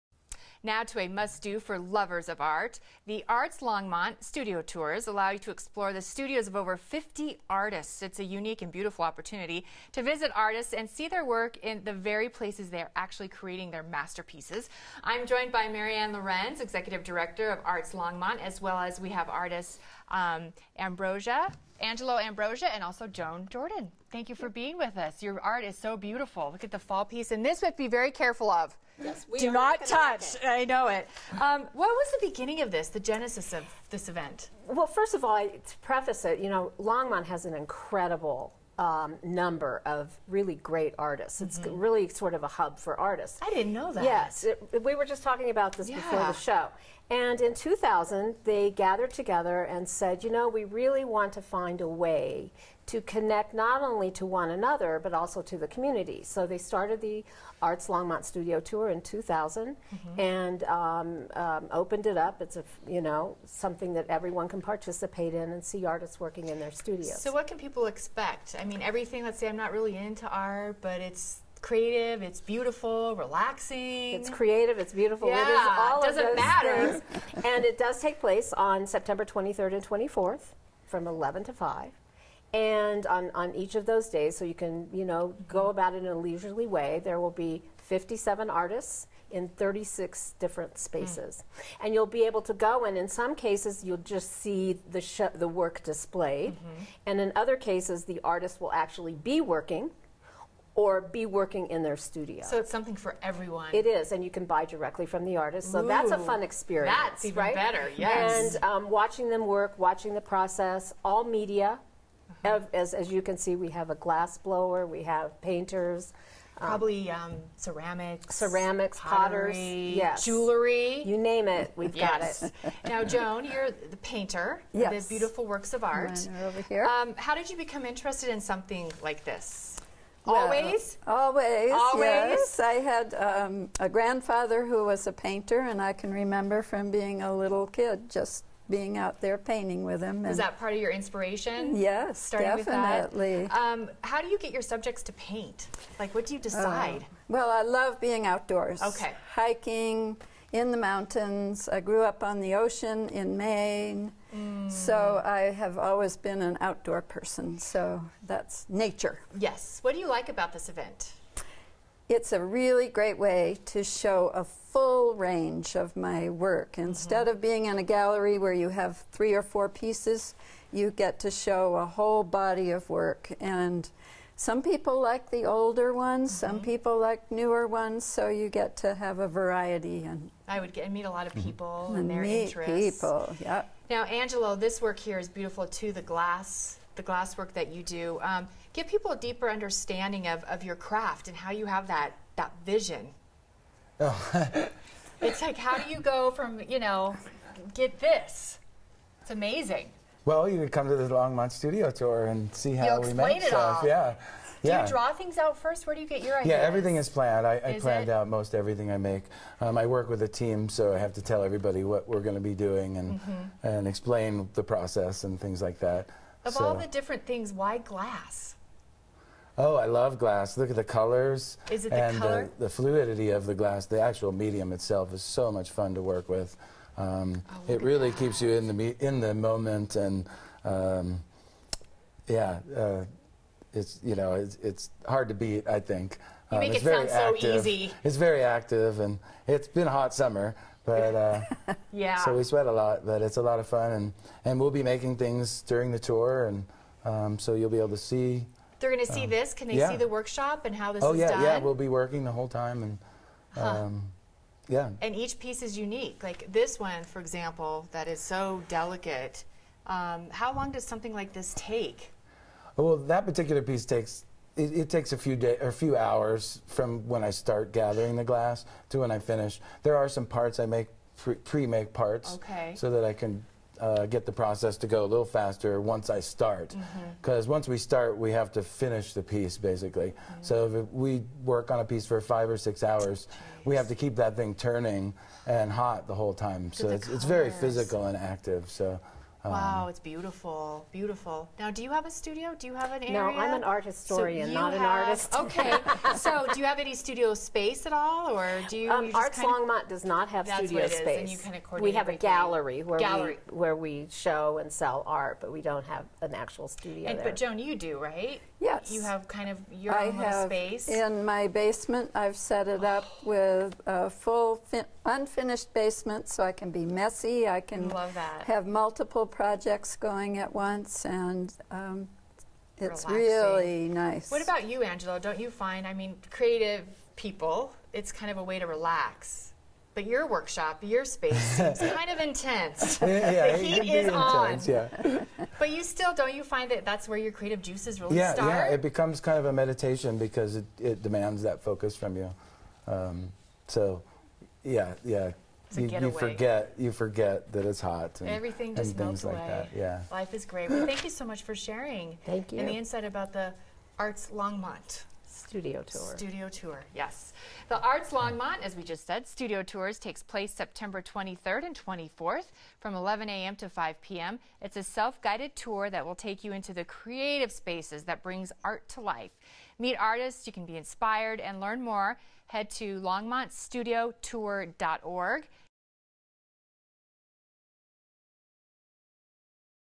Longmont Art Studio Tour - September 15, 2017
The Arts Longmont Studio Tours take place September 23rd and 24th from 11am to 5pm.  It's a self-guided tour that will take you into the creative spaces that brings art to life. Meet artists and be inspired.